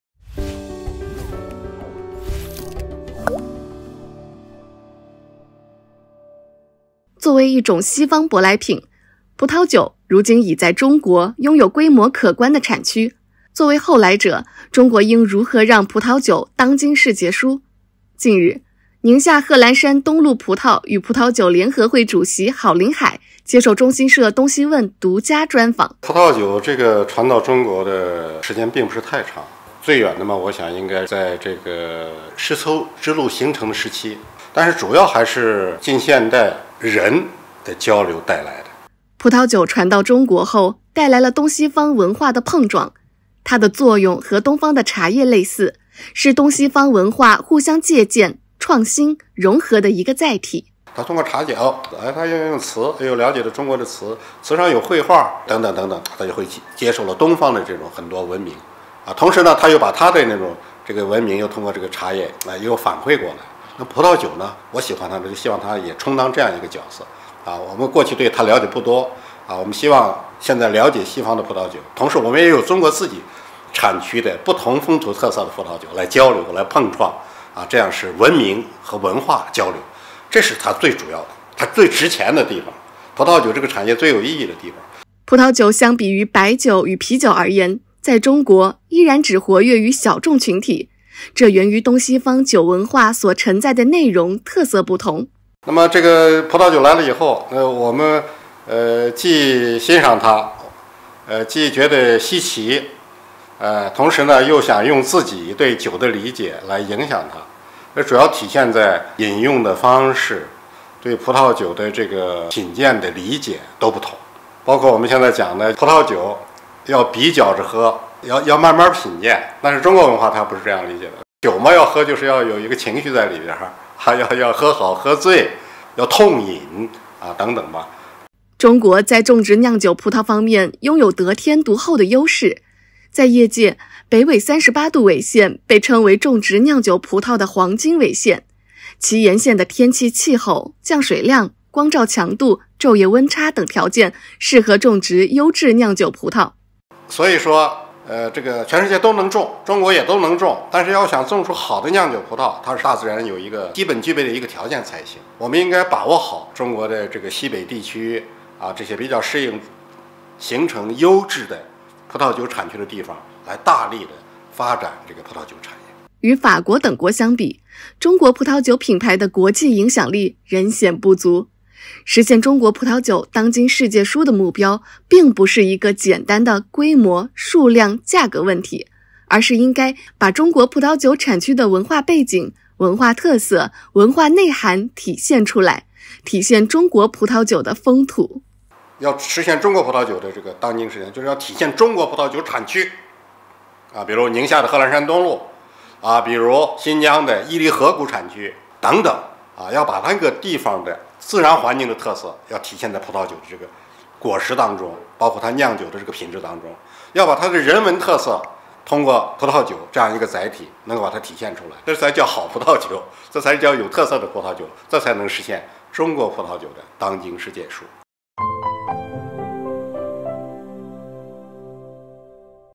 【东西问】中国如何让西方舶来品葡萄酒“当惊世界殊”
葡萄酒传到中国后，带来了东西方文化的碰撞，它的作用和东方的茶叶类似，是东西方文化互相借鉴、创新、融合的一个载体。
作为一种西方舶来品，葡萄酒如今已在中国拥有规模可观的产区，作为后来者，中国应如何让葡萄酒“当惊世界殊”？近日，宁夏贺兰山东麓葡萄与葡萄酒联合会主席郝林海接受中新社“东西问”独家专访。